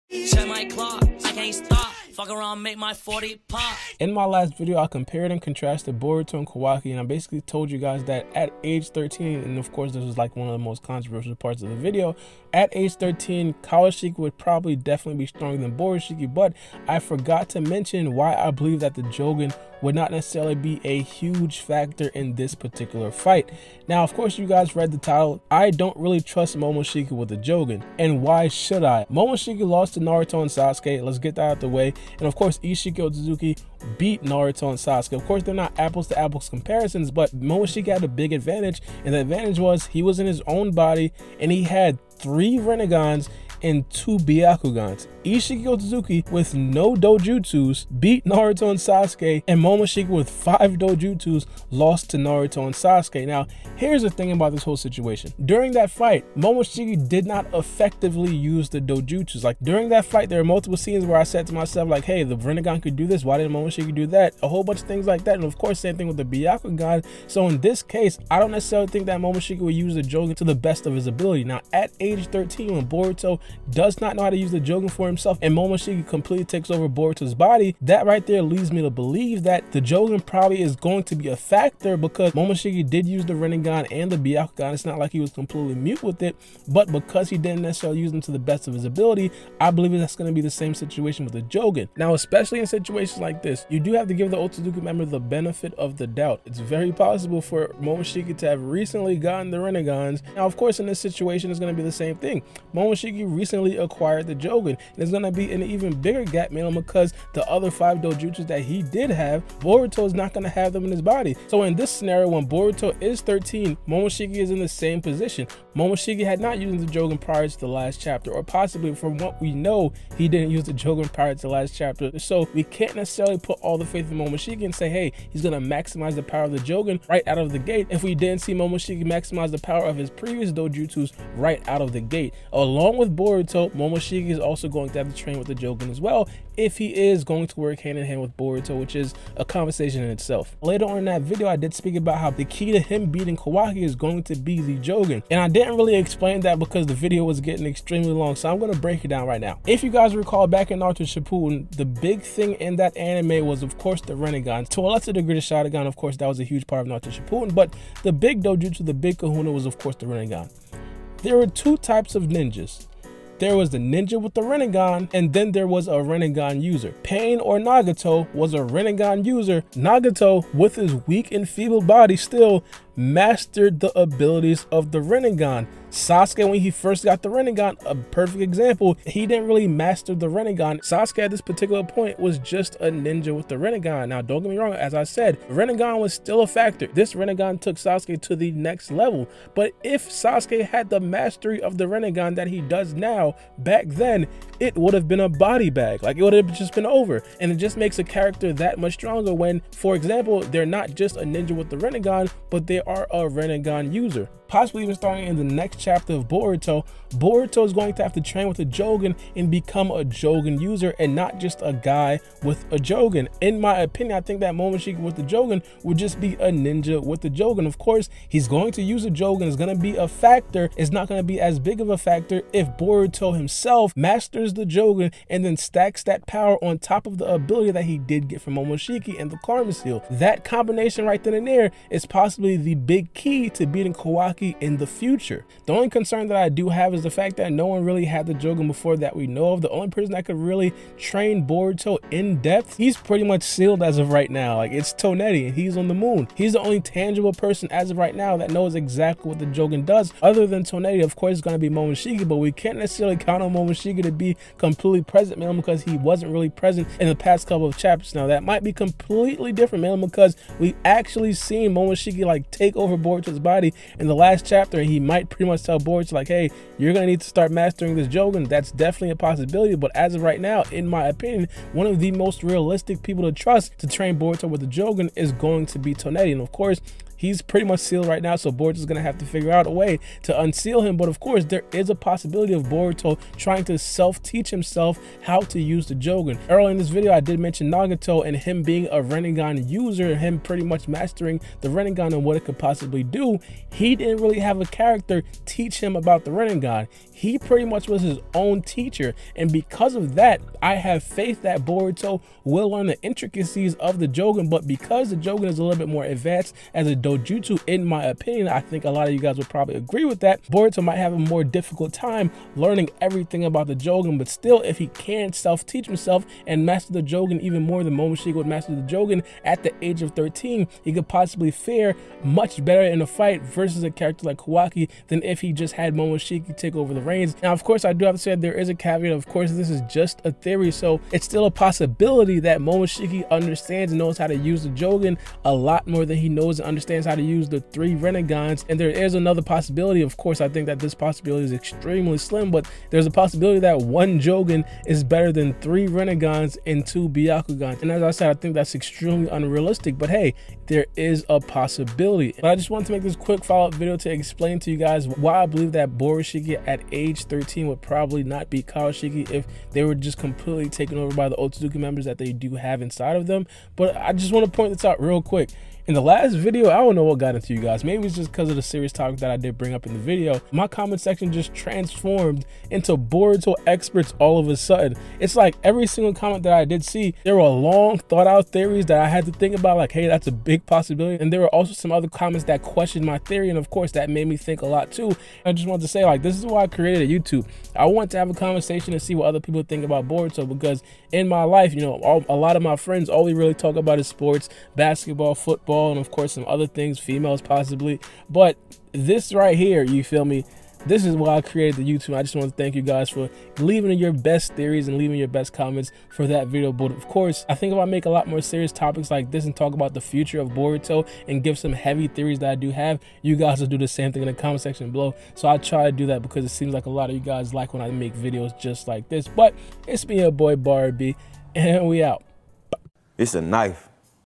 Why I Don’t Trust Momoshiki With the Jougan Yet
In this video I talk about why I believe momoshiki just taking over for boruto won’t be good enough to beat isshiki Ōtsutsuki in kawaki as of right now. The jougan is going to have to be a key to momoshiki and Boruto winning this particular battle. The lack of efficiency of momoshikis dojutsu in the battle vs naruto and Sasuke gives me pause when trying pictures how effective momoshiki will be with the jougan. KawaShiki vs BoruShiki will be the battle that we all want to see after the time skip, but how well is boruto going to hone the karma seal and the jougan to ensure success in the battle.

Do you trust momoshiki right now with karma seal?